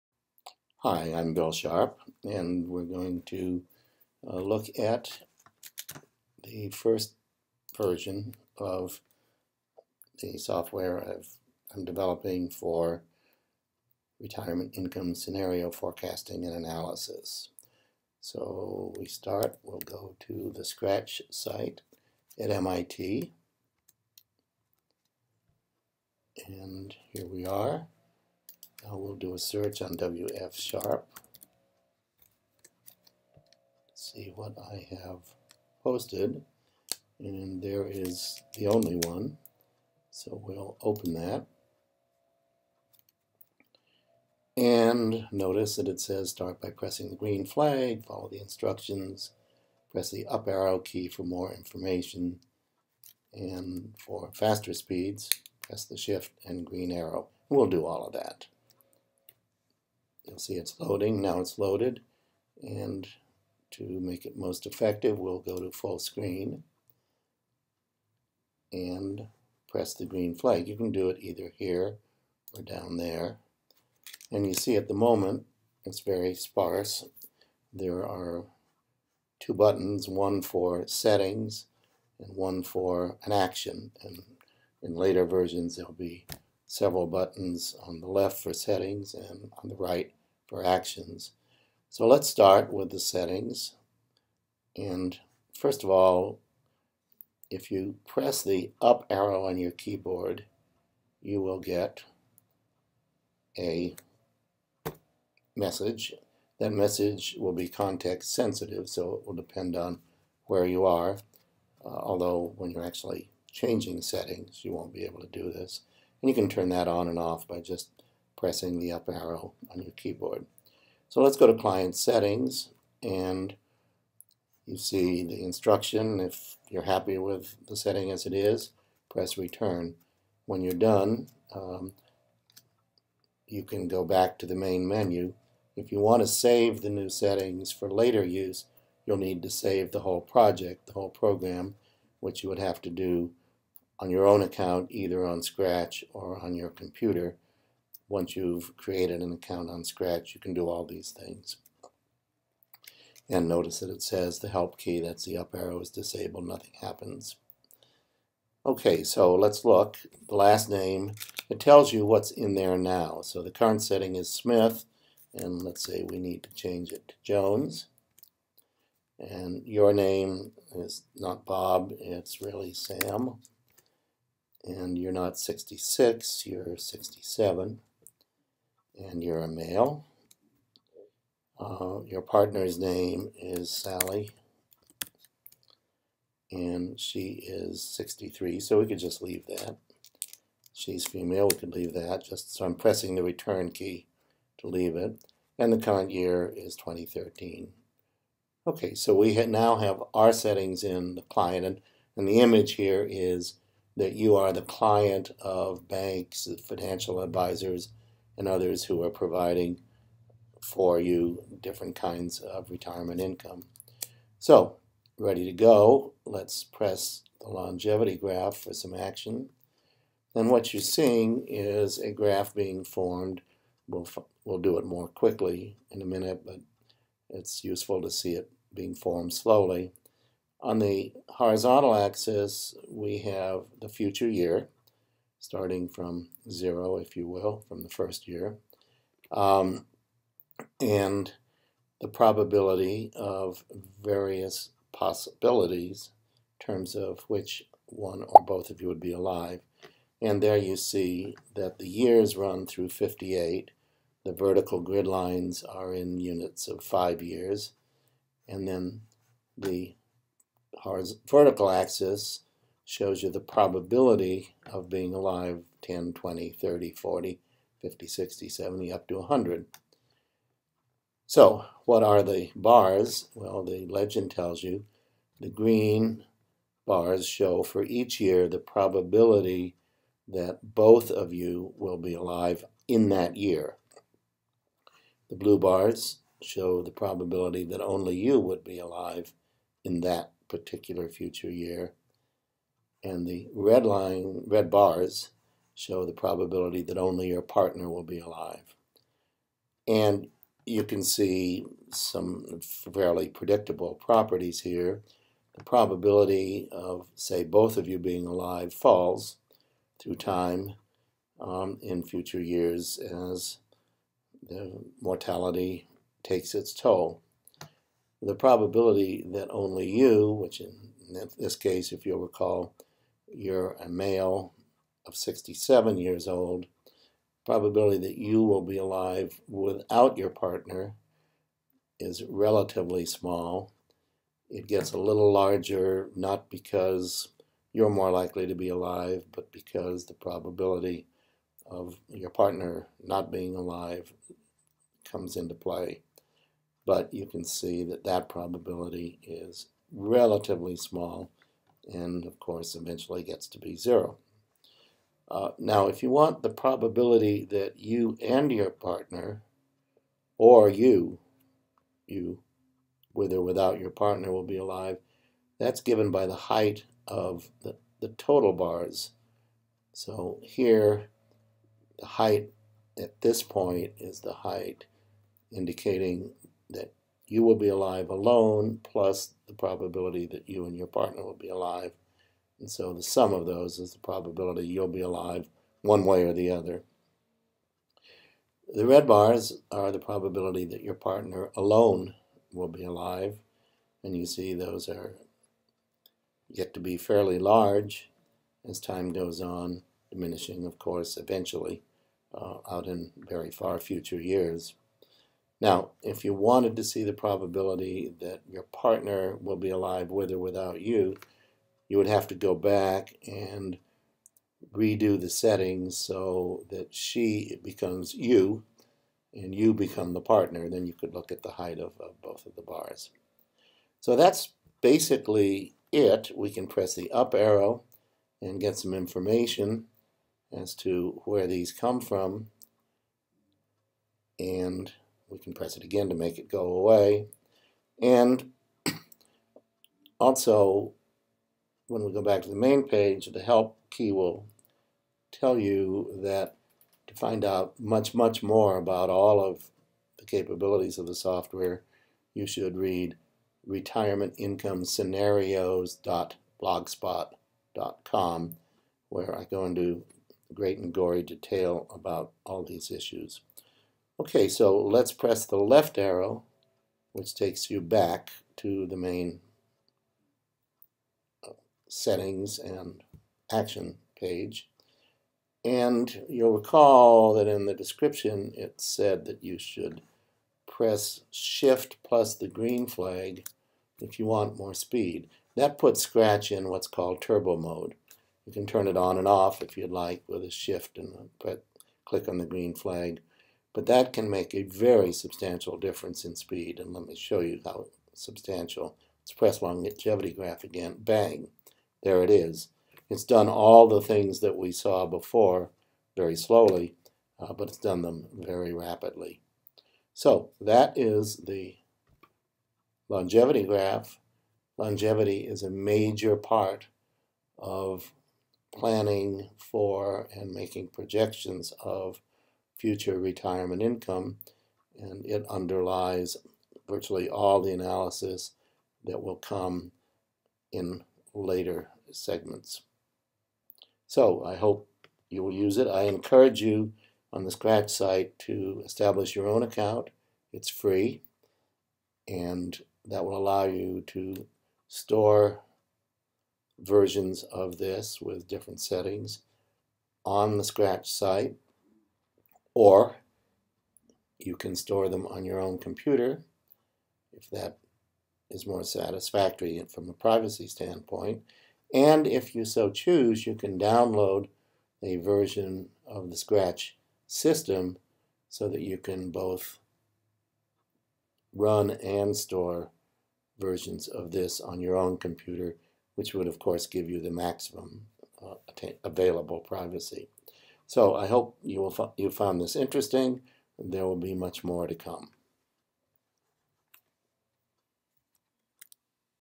Retirement Income Scenarios -- Longevity
An introduction to the RIS (Retirement Income Scenarios) software and the longevity graph function.